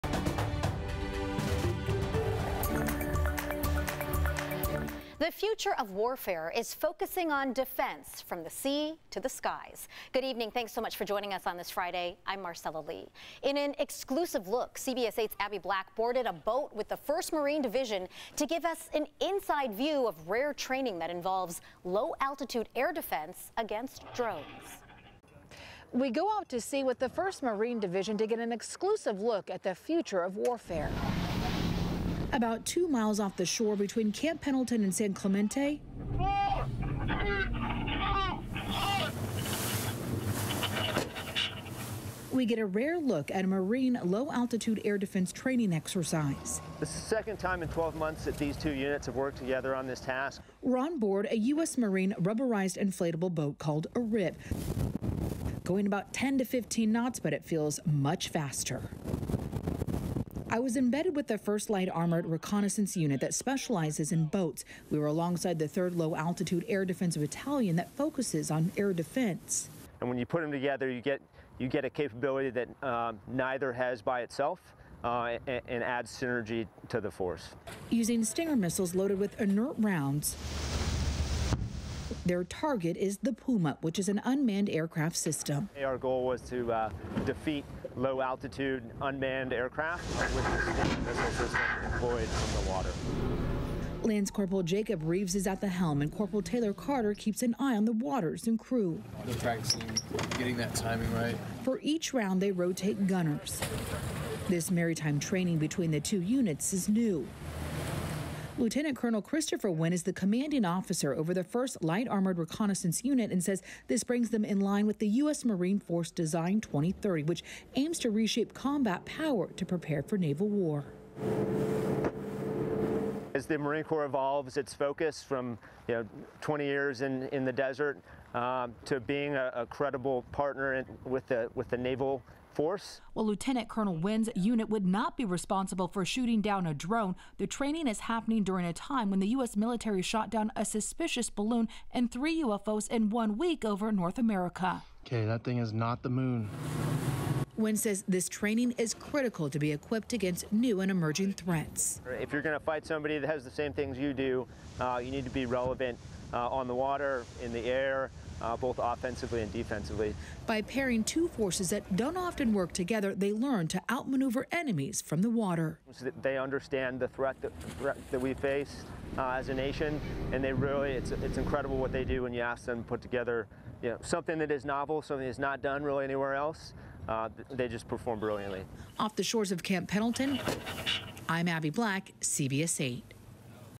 Embedded with the 1st Marine Division | The future of warfare is focusing on defense from the sea
A CBS 8 exclusive: Marines from the 1st Marine Division and 3rd Marine Air Wing join forces in a new maritime exercise involving shooting down unmanned aircraft.